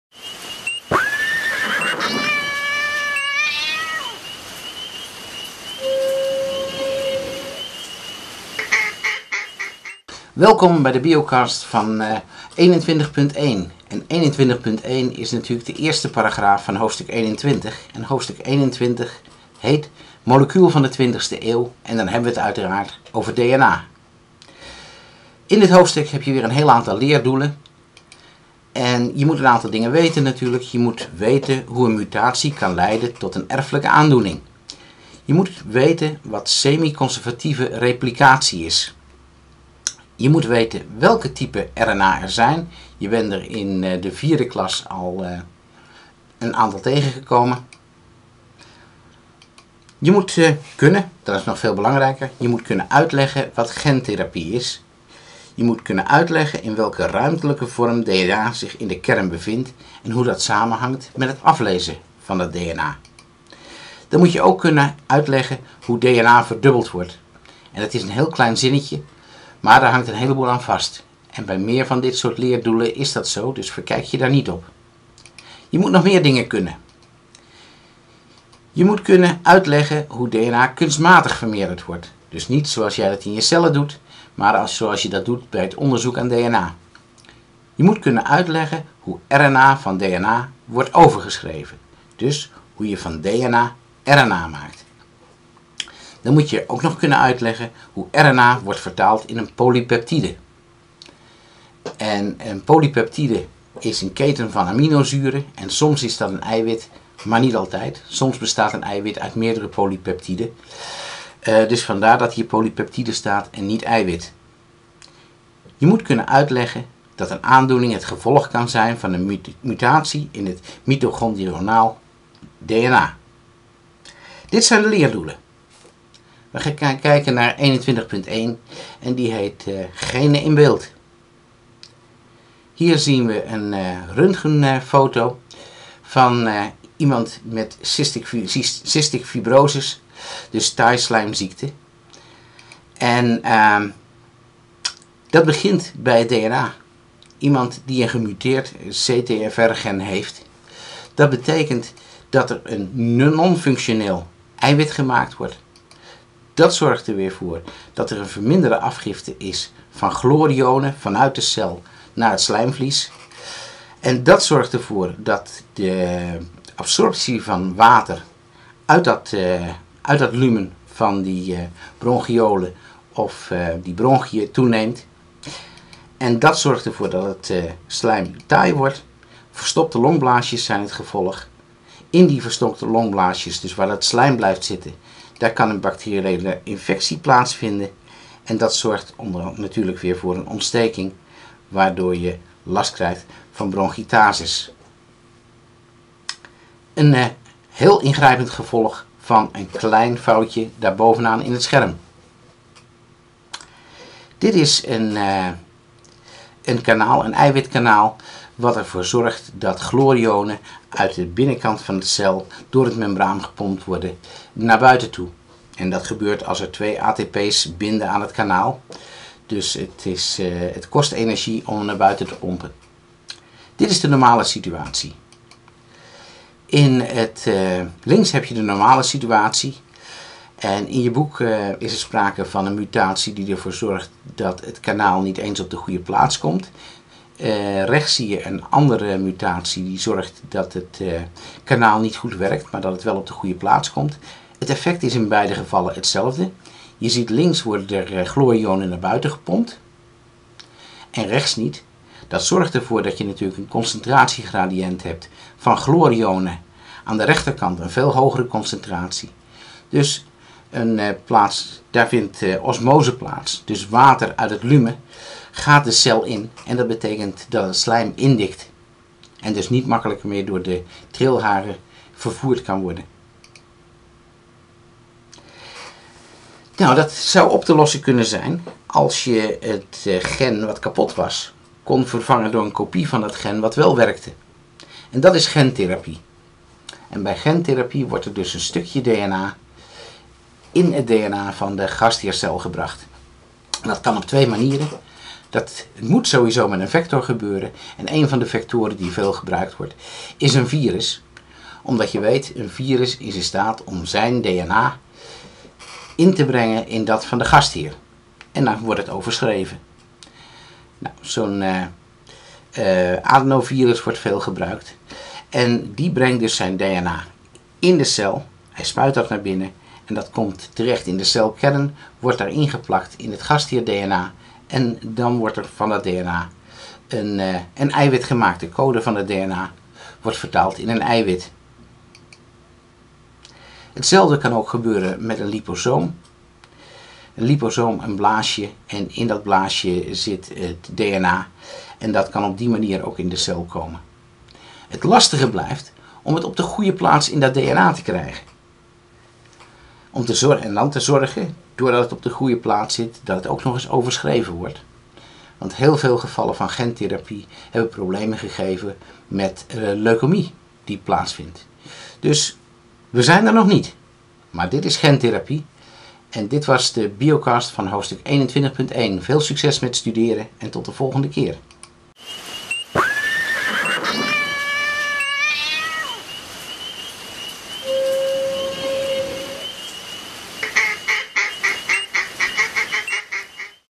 21.1 biocast
Een podcast over Hoofdstuk 21.1 van Nectar voor V6